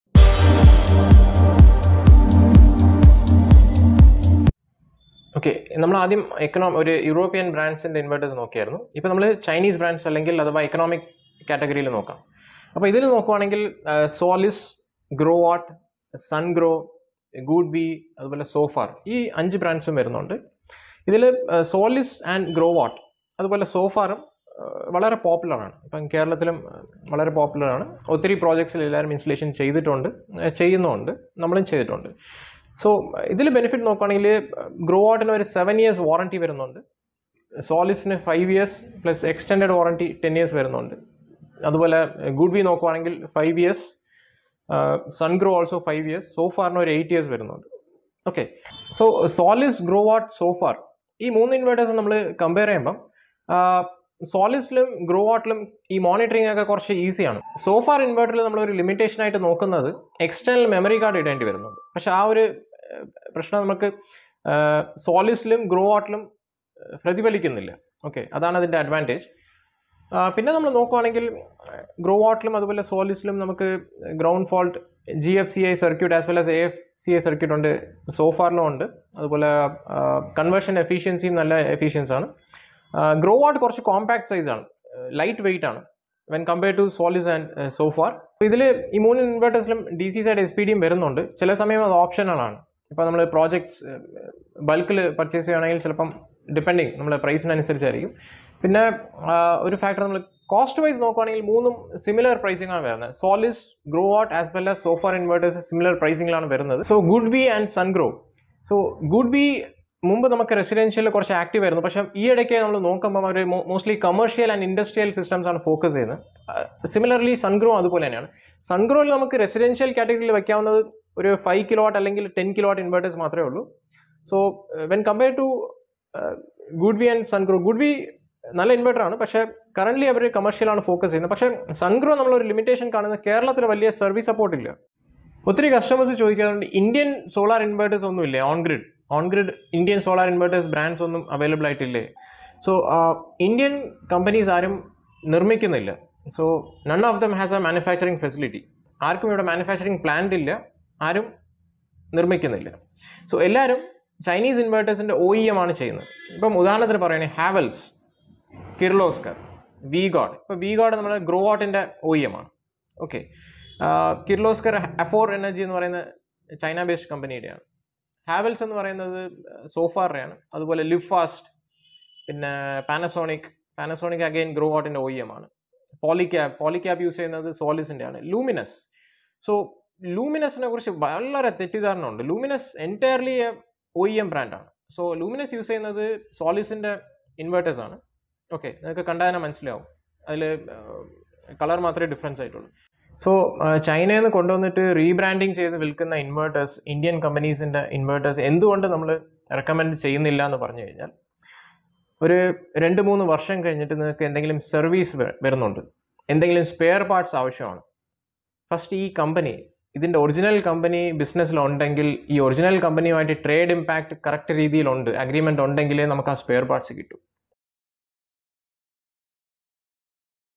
Economic Category Solar On-Grid Inverters |Part-3| Best Performer
⚡ Solis| Growatt| Sungrow| Goodwe| Sofar| -- Economic Chinese Inverter Brands⚡

✅Lets Find out the Best Performing Economic Brand PV Inverter

👉 This Videos is in Malayalam language. There is also English and Hindi Version available.
✅ Comorin Solar is based out of Kerala, India and we do On-Grid and Hybrid Solar Installations.